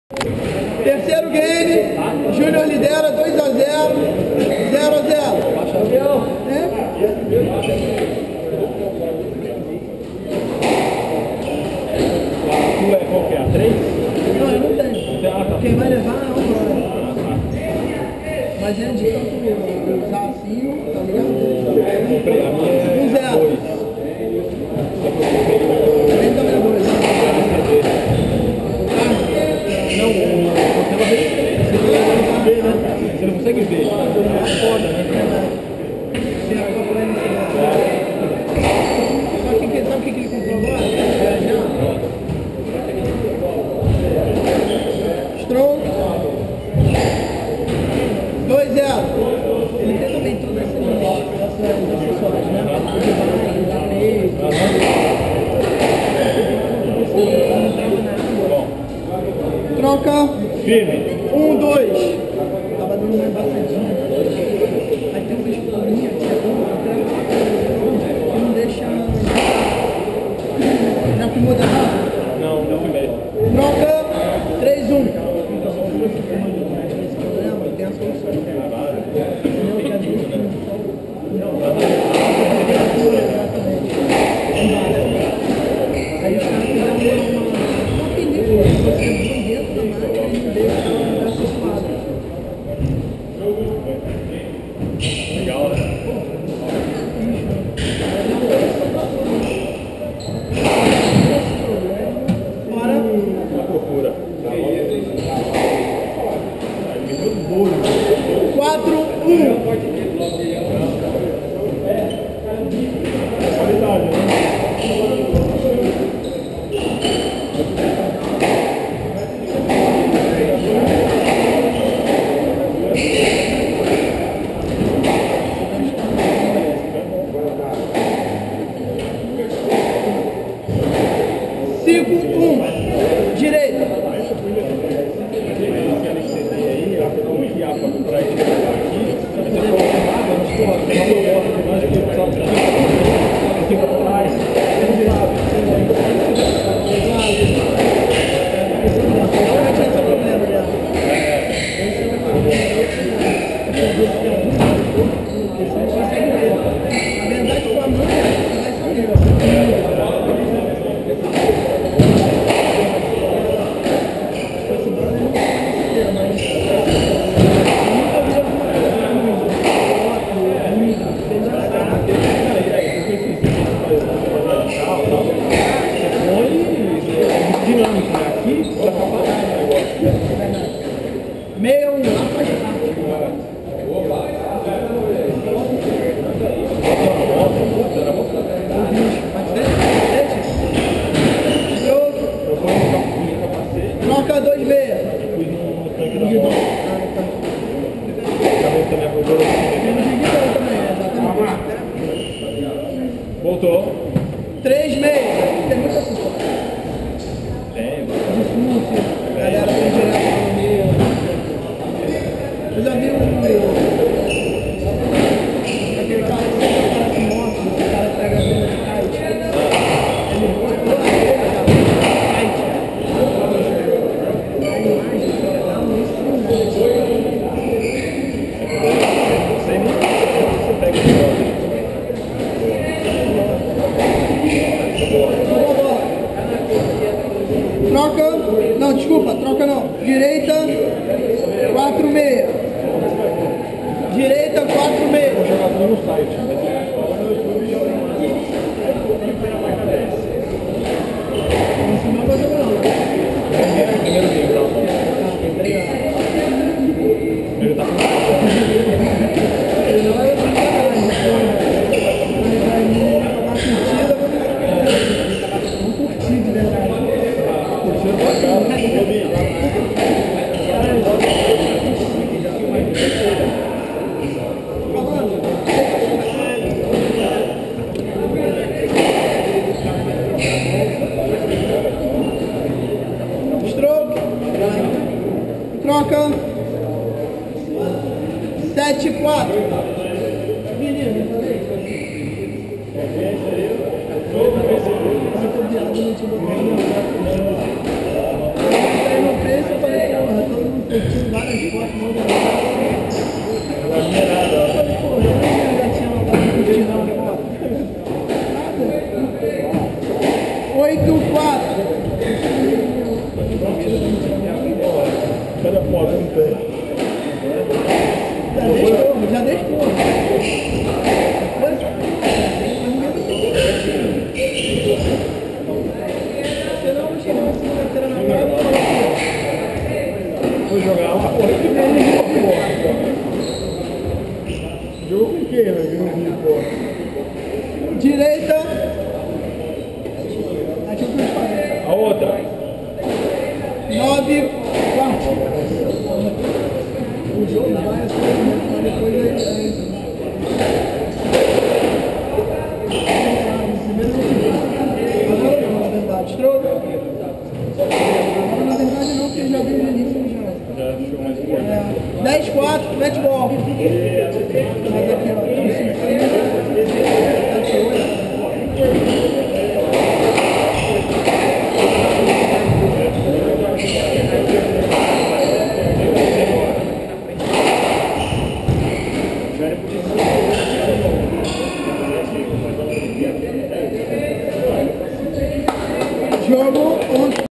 Junior Christovam x José Pedro Escobar
Jogo válido pelas quartas de final do torneio APS Squash em Campinas 2013.